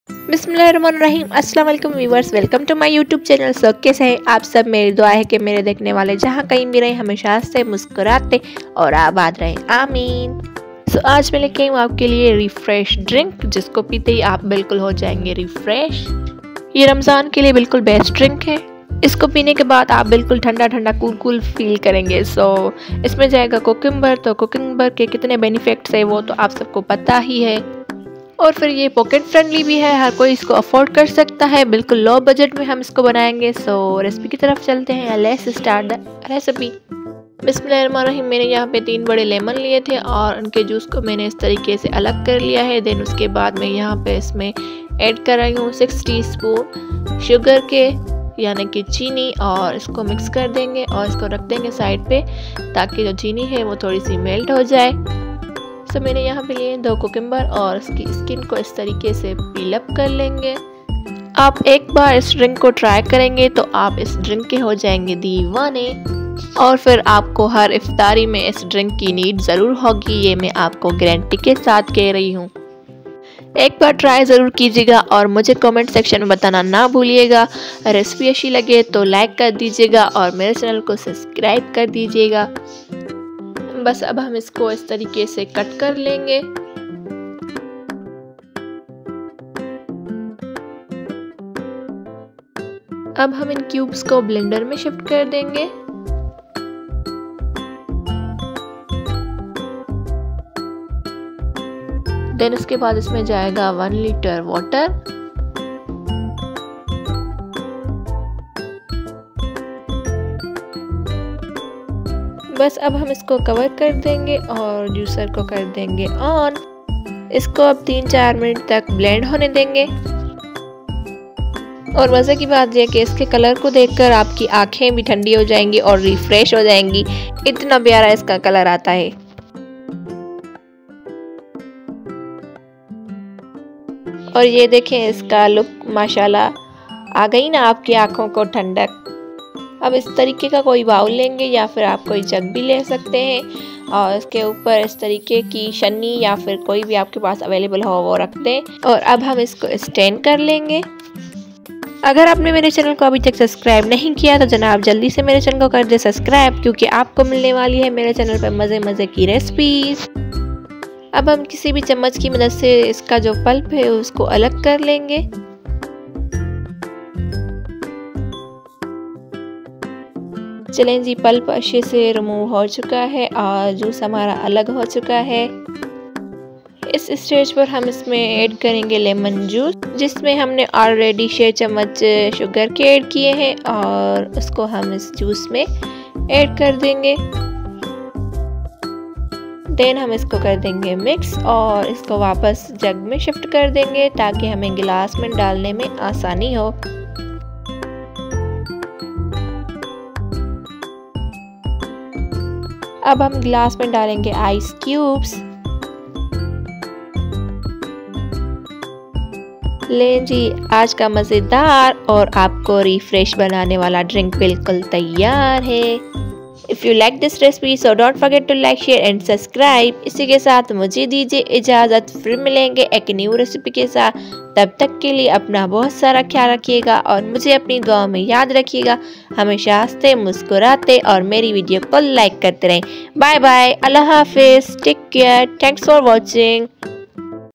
Most Refreshing Drink by TipdishFood | Cucumber Drink For the Hot season | Iftar Drink Recipe
how to make Afghani Omelet Recipe